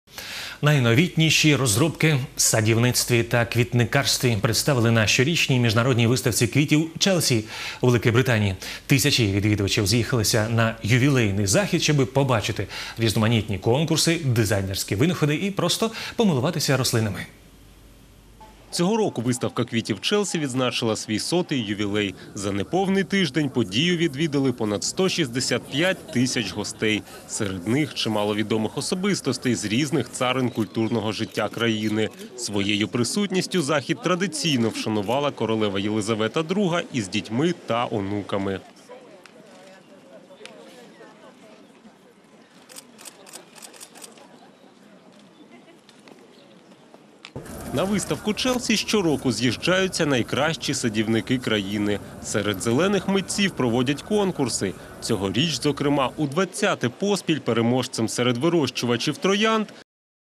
Садівниче шоу в Челсі: 165 тисяч глядачів та візит королеви Єлизавети
Садівниче шоу в Челсі відкрила королева Єлизавета. Заходу вже принаймні сто років.
Переможців нагороджуватимуть по-англійськи врочисто